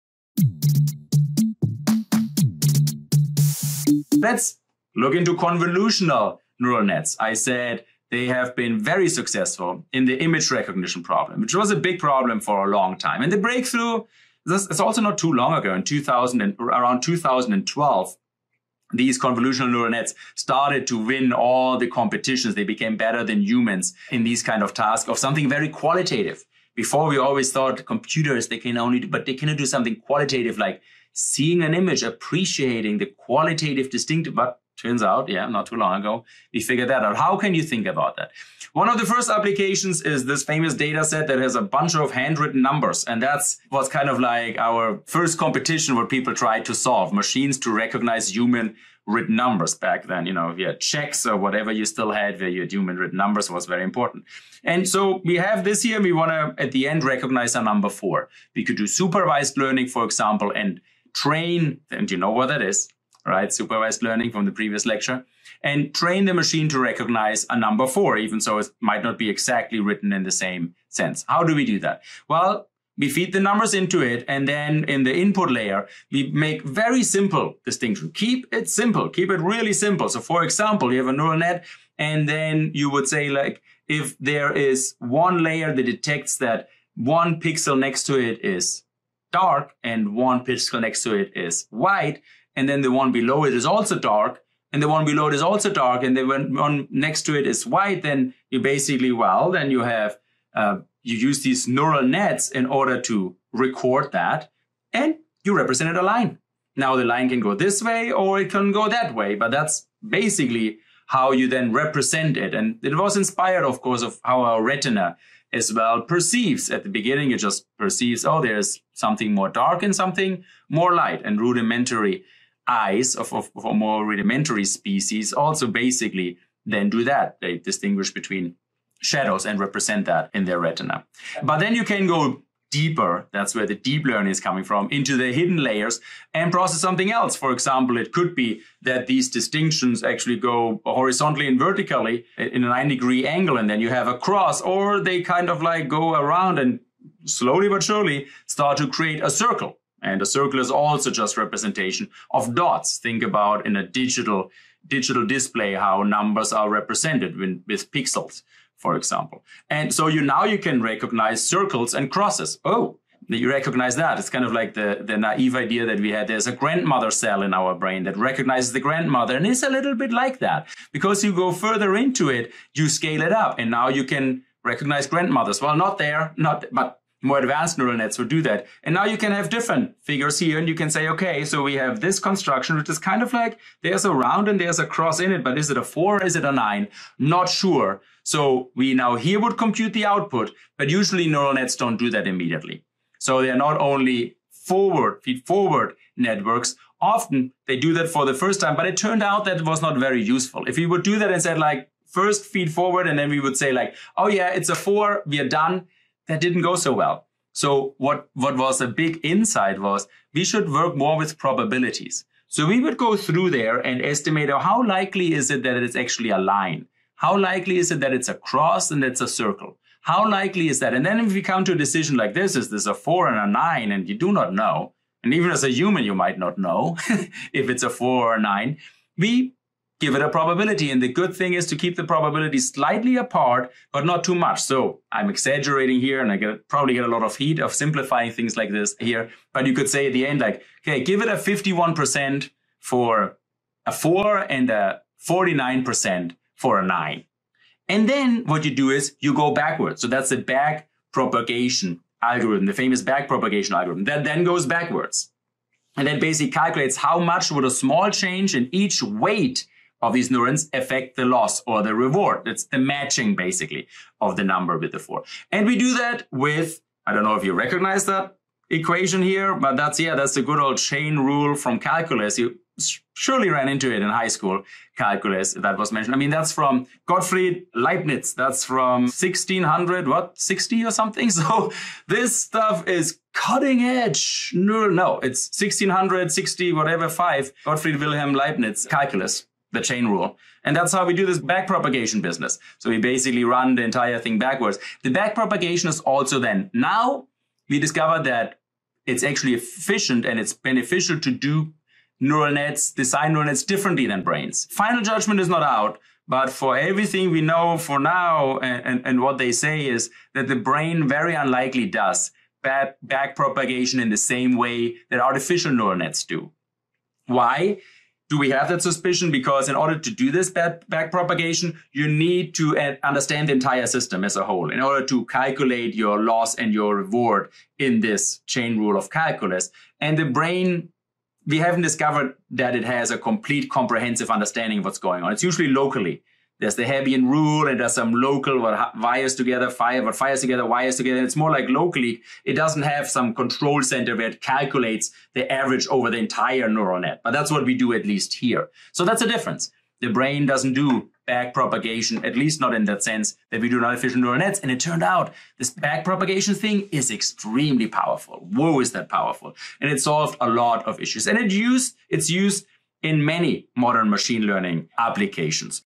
Convolutional Neural Nets (CNN) & Backpropagation = How Machines Learned to See
💡 Machine learning has revolutionized fields like image recognition in recent years. But did you know that techniques like convolutional neural networks rely on mathematical methods developed centuries ago? This video explores how concepts like backpropagation and the chain rule allow neural nets to teach themselves to recognize images. It's amazing how cutting edge AI utilizes age-old calculus to "see" the world!
📚 Quick Lesson: Understanding these core mechanisms can demystify AI and illuminate how the technology achieves its results. Check out the video for an accessible overview of the math powering image recognition.

This snippet is taken from the 2023 version of the University of California online course called "Digital Technology & Social Change".